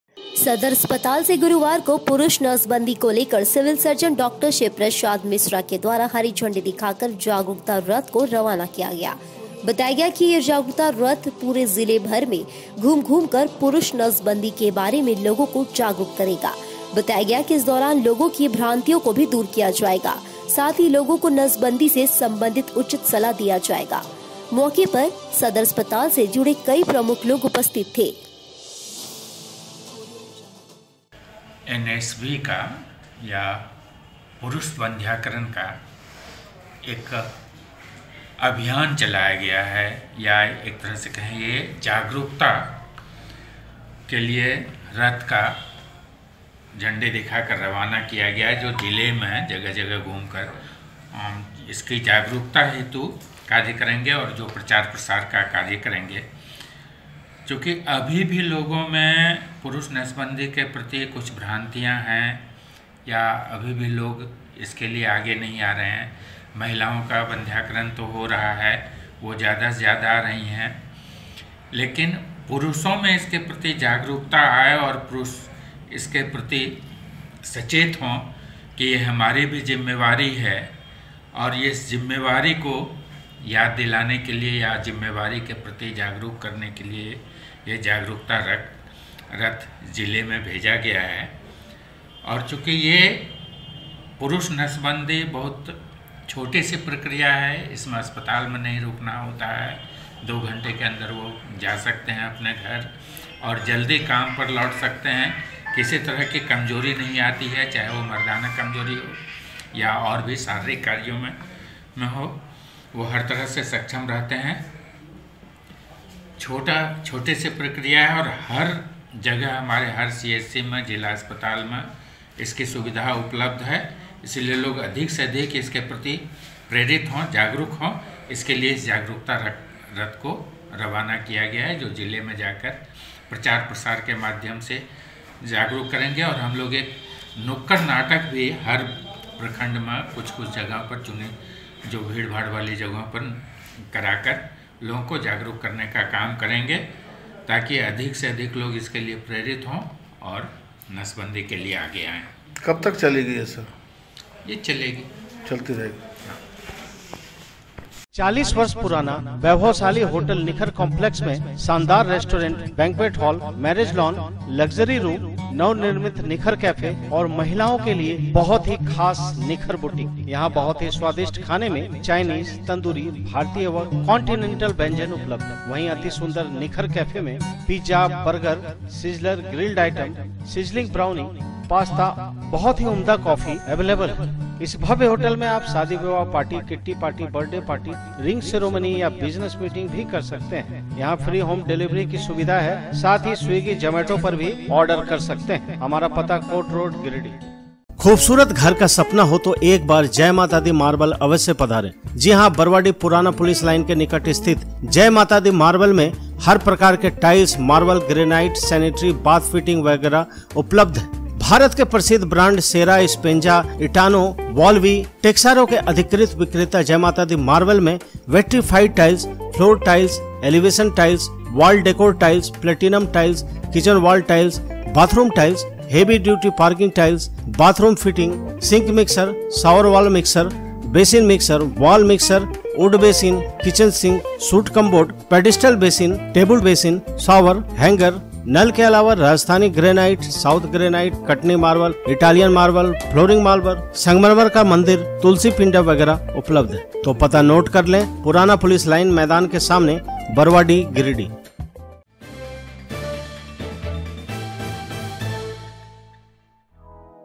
Giridih पुरुष नसबंदी को लेकर सिविल सर्जन के द्वारा रवाना किया गया जागरूकता रथ
सदर अस्पताल से गुरुवार को पुरुष नसबंदी को लेकर सिविल सर्जन डॉ शिव प्रसाद मिश्रा के द्वारा हरी झंडी दिखाकर जागरूकता रथ को रवाना किया गया।